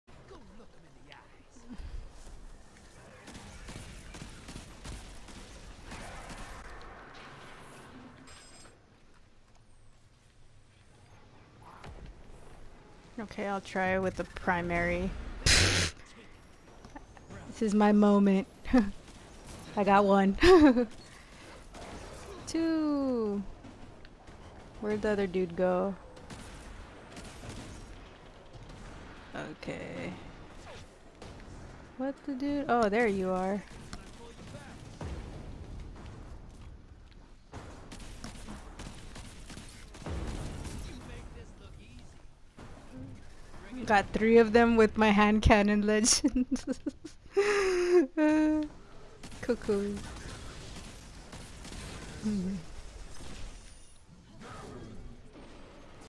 Invading With Malfeasance
So I took a chance on Invading without Heavy. While heavy isn’t necessary for invading, I prefer using Power Ammo. These blueberries got caught up in adds but I still got them. The perk for Malfeasance is nice in killing Invaders and Taken. Ace of Spades ♠️ is a great gun to invade with as well and shotguns can help out too. I need to practice on more long ranged weapons heh. But here’s a primary weapon invade. :)